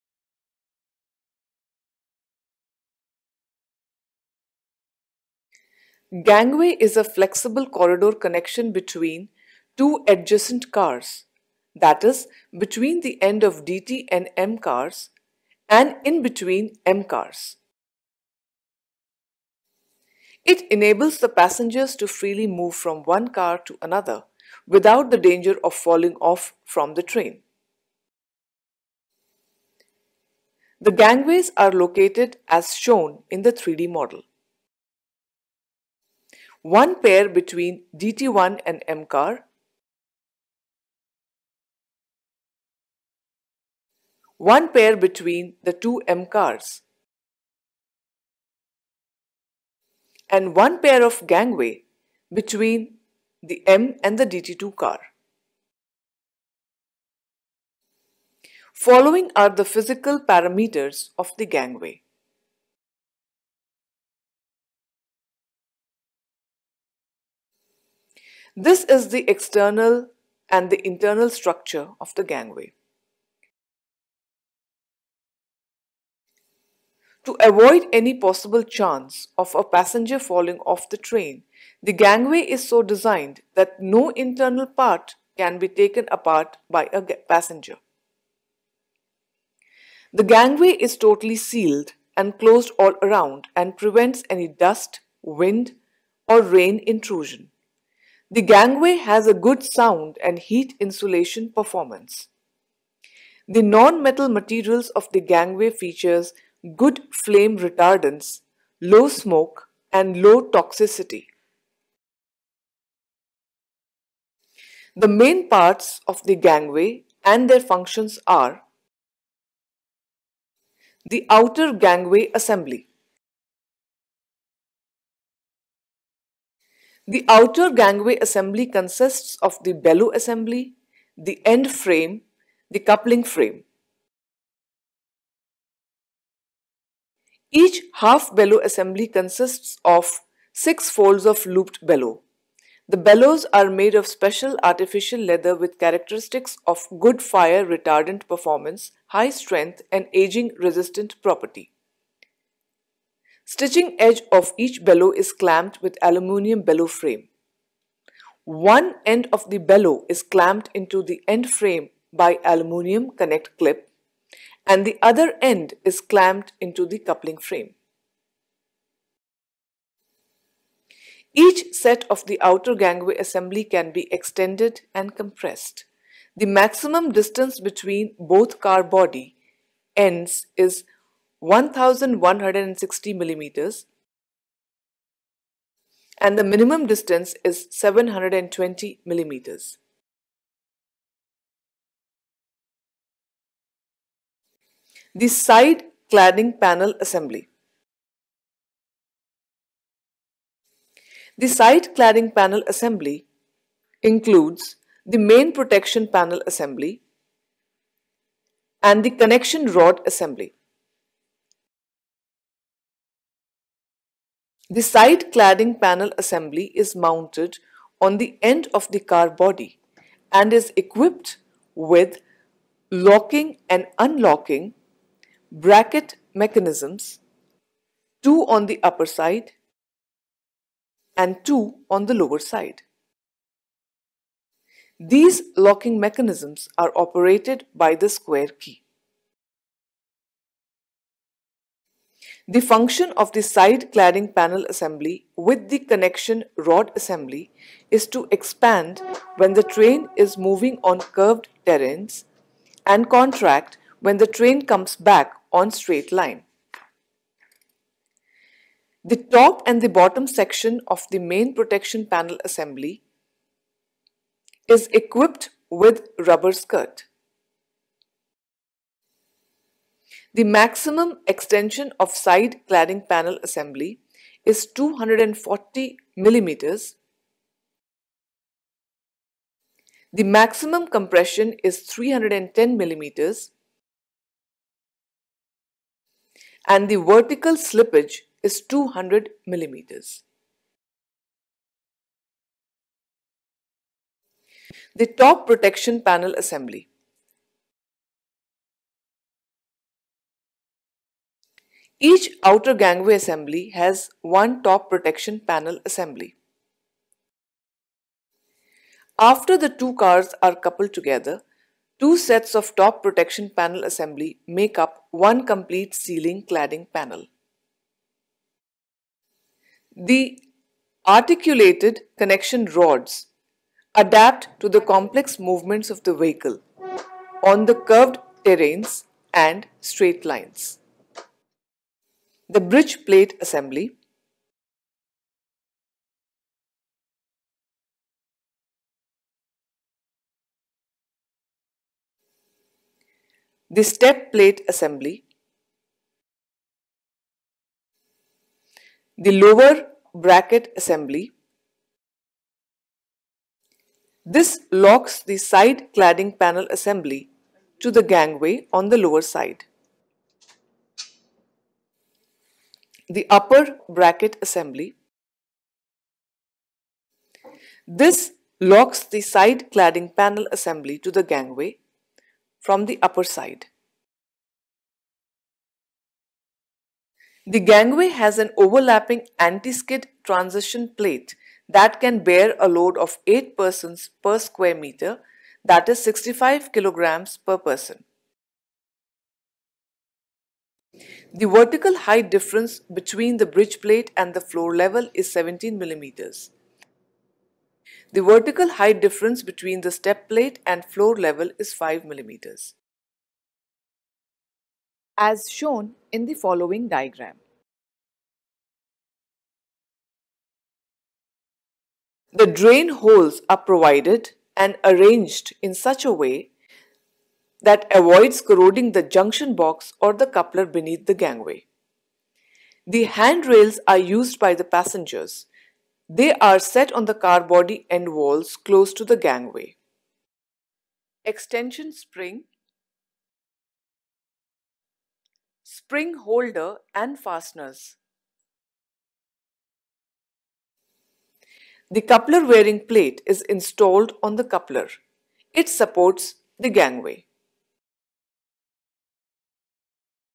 Introduction to Parts of Gangway assembly
This is 3D Animation video, We have described Parts of Gangway assembly. All content and visuals have been thoroughly approved by Metro Rail officials at all levels .